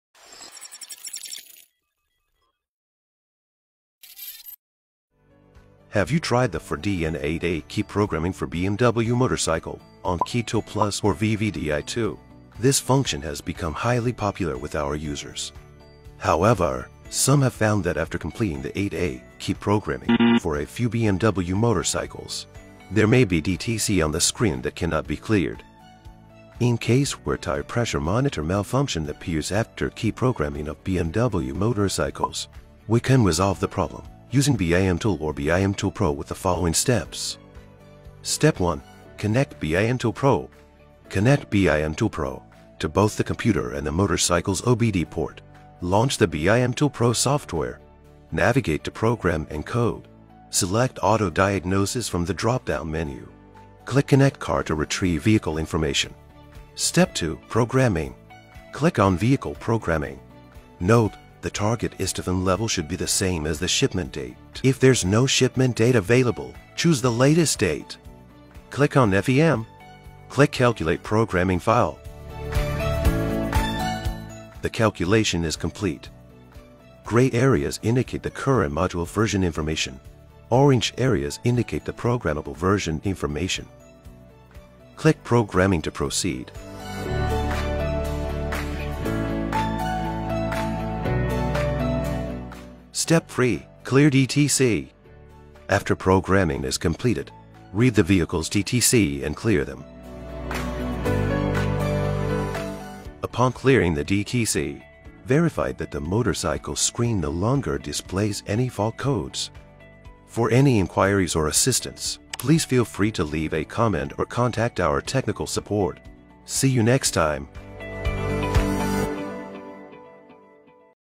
How to solve BMW Motorcycle 4D 8A TPMS Malfunction Error with VVDI BIMTool Pro - xhorse.co.uk
This video shows you how to solve BMW Motorcycle 4D 8A TPMS Malfunction Error with Xhorse VVDI BIMTool Pro.